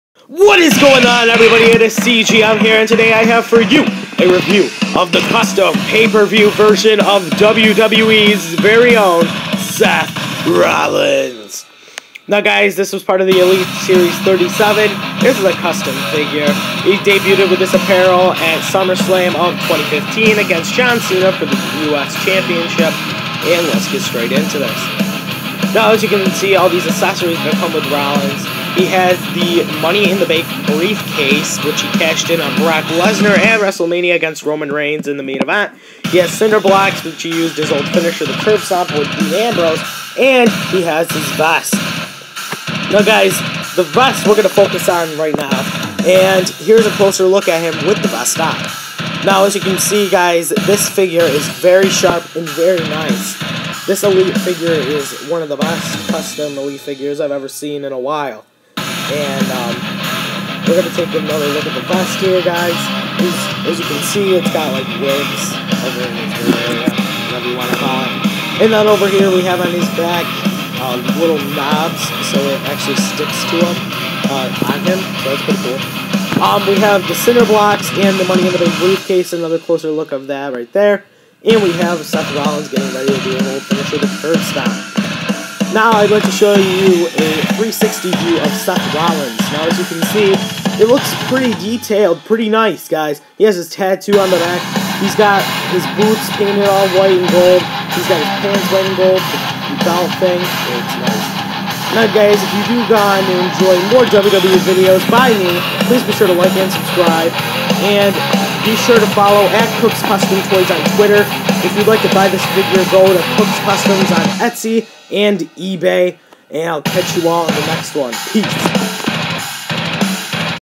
WWE ELITE CUSTOM SETH ROLLINS SUMMERSLAM 2015 FIGURE
Hey guys this is a custom Seth Rollins figure Elite with his SummerSlam 2015 outfit! Be sure to follow @CookzCustomToyz on Twitter and check out Cookz Customz on EBay and Etsy if you'd be interested in buying this figure for yourself!